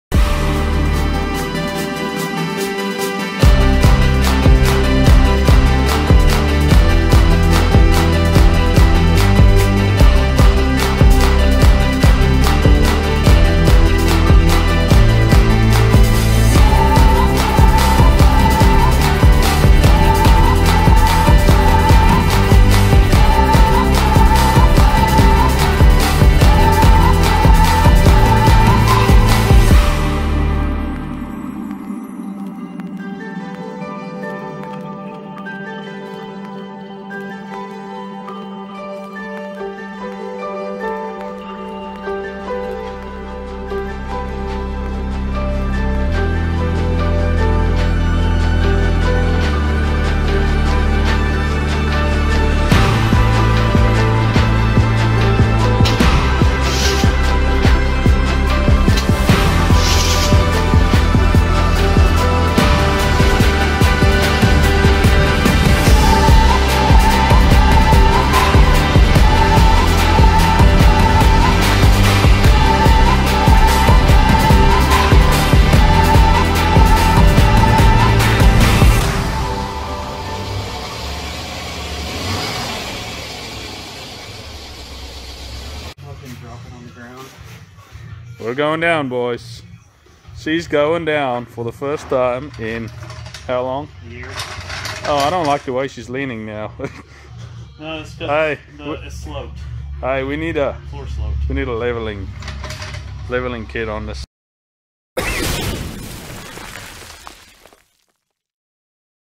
Junkyard motor install
One of my friends bought a 1997 Chevy silverado 1500 with a locked up engine (5.7 vortec) and he ended up not "finishing his project" , in return I needed a pickup at the time and with the help of my friend Tyler we pulled a motor out of a wrecked 1998 Chevy and just replaced all the seals before we installed it! a Week of long cold winter work nights later had a running driving truck!